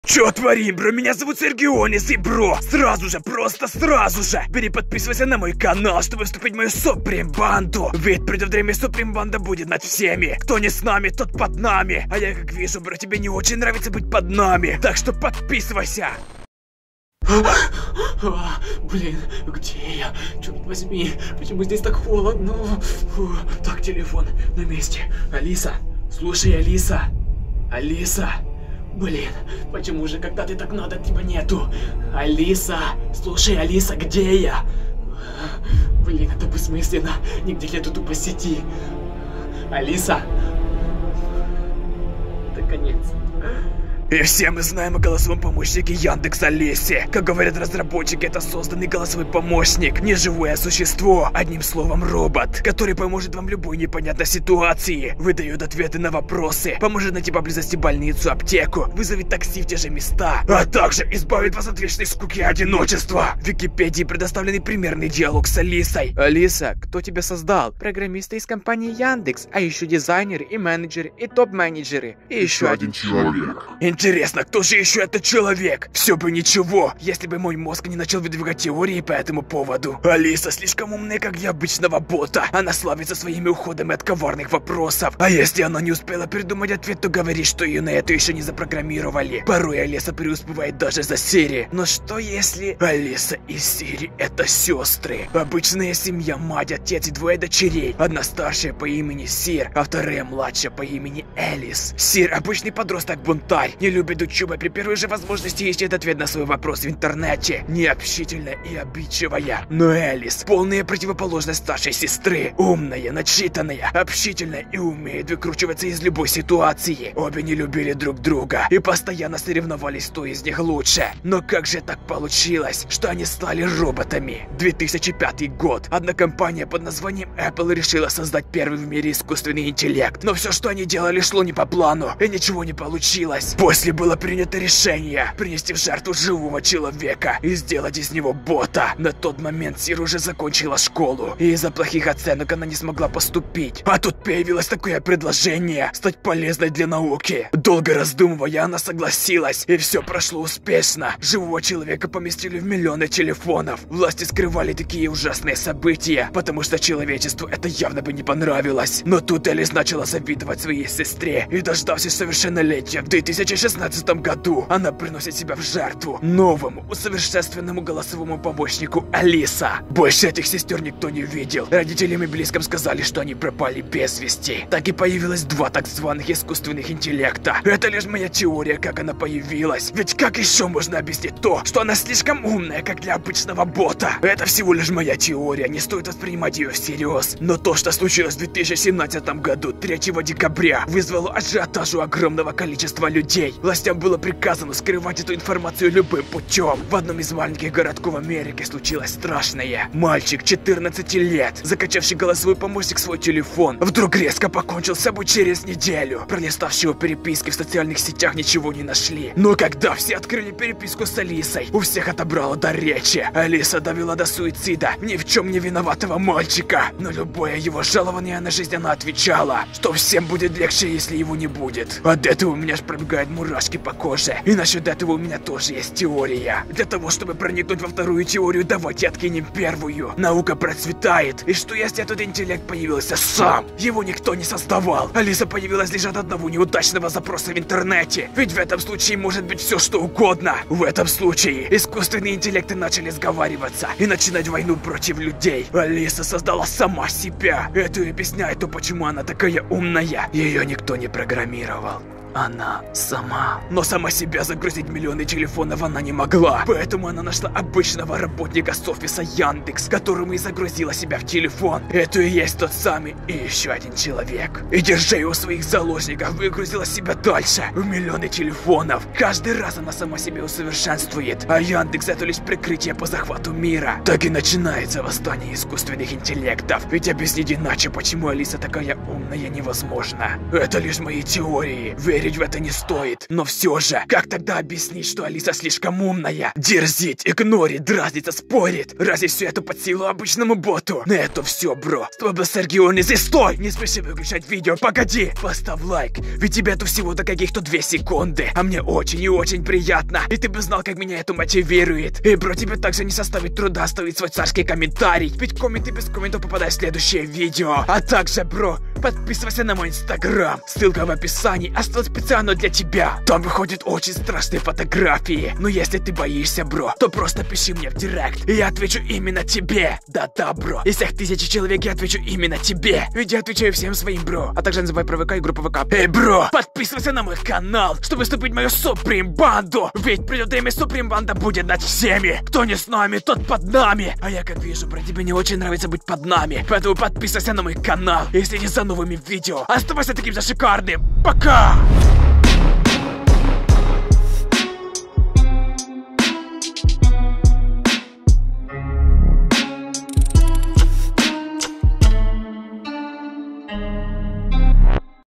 ТЕМНАЯ СТОРОНА АЛИСЫ | ЯНДЕКС | YANDEX
Моя жуткая теория по поводу той самой зловещей Алисы

СПРАШИВАЙ: ask.fm/s_e_r_h_y_i__k_i_n_g

ЯНДЕКС алиса YANDEX